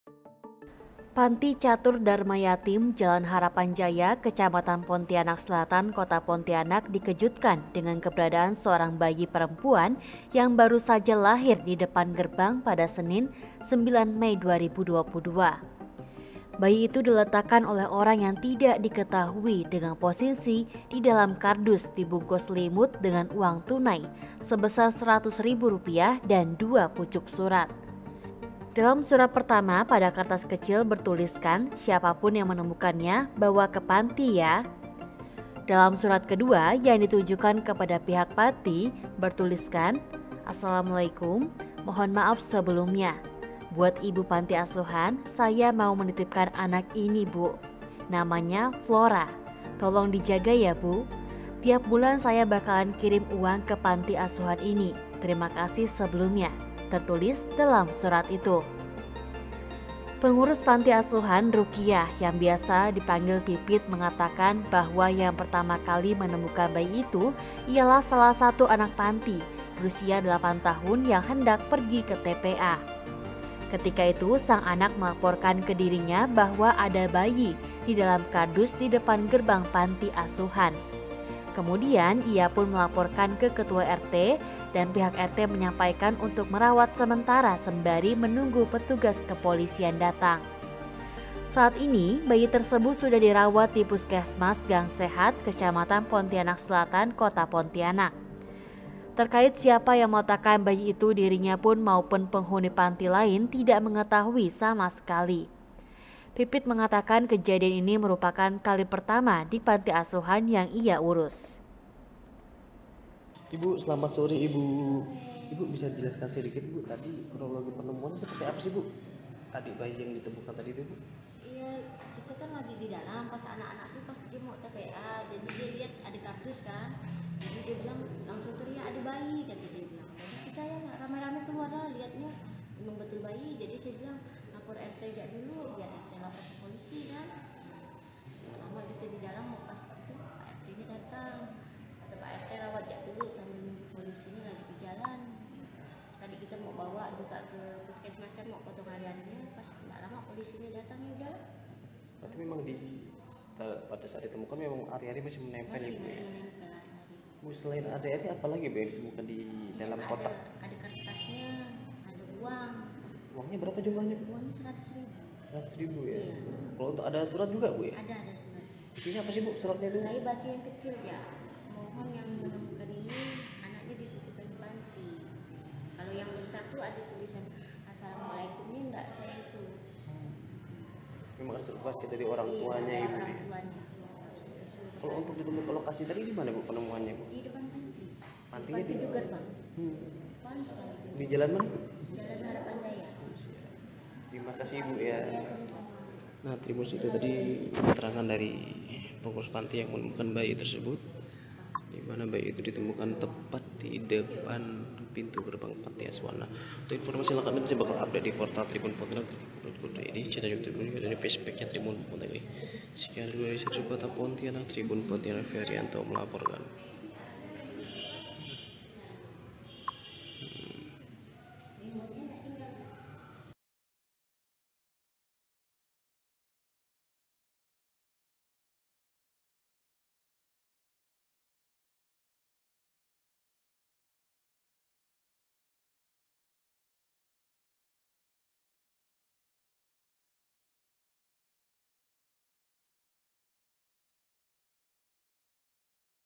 🔴 Isi Surat Dalam Kardus Bayi yang ditemukan Di Depan Panti Asuhan Catur Dharma Yatim Pontianak
Panti Catur Dharma Yatim, Jalan Harapan Jaya, Kecamatan Pontianak Selatan Kota Pontianak dikejutkan dengan keberadaan seorang bayi perempuan yang baru saja lahir di depan gerbang pada senin 9 Mei 2022.

Bayi itu diletakkan oleh orang yang tidak diketahui dengan posisi di dalam kardus dibungkus selimut dengan uang tunai sebesar seratus ribu rupiah dan dua pucuk surat

Dalam Surat Pertama pada kertas kecil Bertuliskan
"Siapapun yang menemukannya, bawa ke Panti ya".

Dalam Surat Kedua yang ditujukan kepada pihak Panti
"Assalamualaikum. Mohon maaf sebelumnya, buat ibu panti asuhan, saya mau menitipkan anak ini Bu, namanya Flora, tolong dijaga ya Bu, tiap bulan saya bakalan kirim uang ke Panti Asuhan ini, Terima kasih sebelumnya,"tertulis dalam surat itu.

Pengurus Panti Asuhan, Rukiah yang biasa dipanggil Pipit mengatakan bahwa yang pertama kali menemukan bayi itu ialah salah satu anak Panti berusia 8 tahun yang hendak pergi ke TPA.

Ketika itu sang anak melaporkan ke dirinya bahwa ada bayi di dalam kardus di depan gerbang Panti Asuhan.

Kemudian, iapun melaporkan ke Ketua RT, dan pihak RT menyampaikan untuk merawat sementara sembari menunggu petugas kepolisian datang.
Saat ini, bayi tersebut sudah dirawat di Puskesmas Gang Sehat,
Kecamatan Pontianak Selatan, Kota Pontianak.

Terkait siapa yang meletakkan bayi itu dirinya maupun penghuni Panti lain tidak mengetahui sama sekali.

Pipit Mengatakan Kejadian ini merupakan kali pertama di Panti Asuhan yang ia urus.